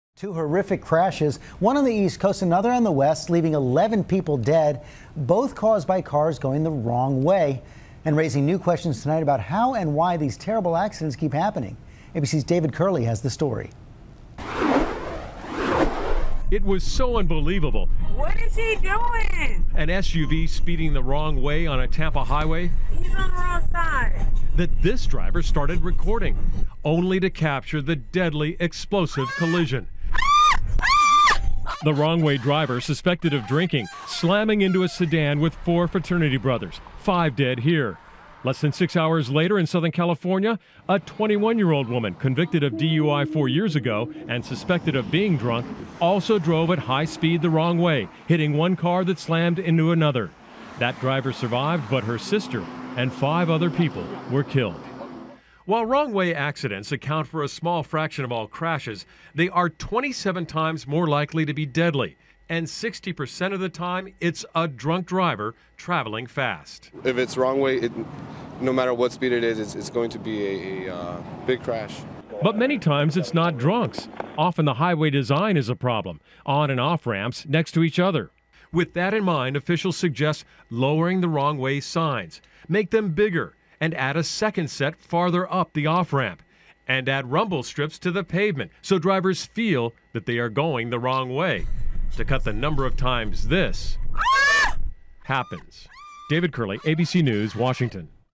11 Dead in Two Horrific Wrong-Way Crashe
Drinking may have been a factor in two deadly crashes in Tampa and Southern California.